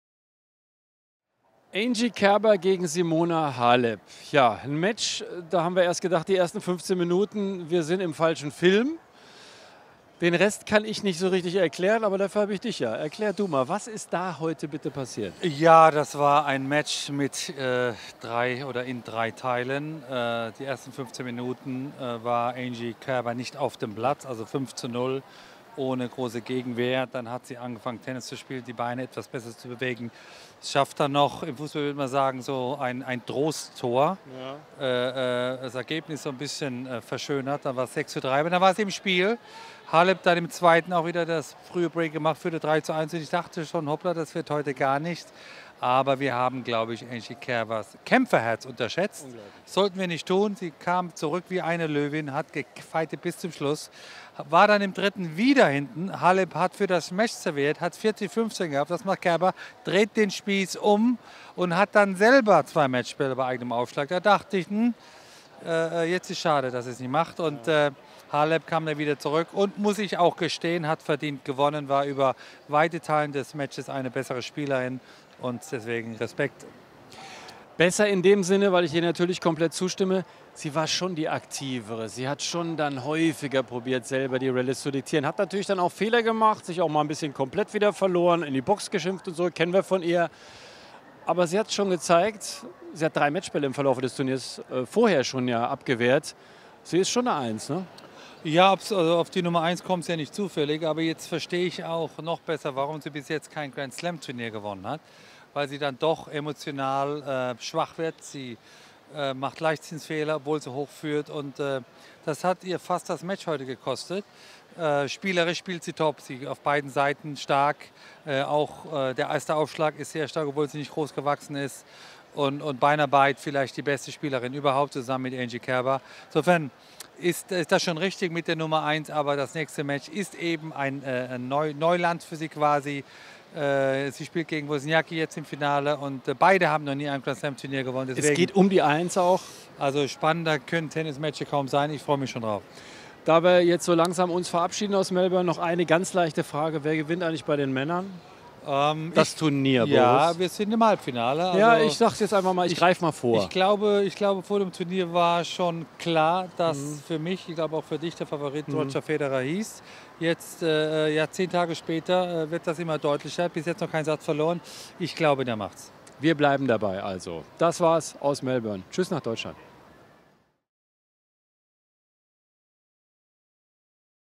Becker zum Kerber-Aus in Melbourne
Boris Becker über das epische Halbfinale der Australian open 2018 zwischen Angelique Kerber und Simona Halep.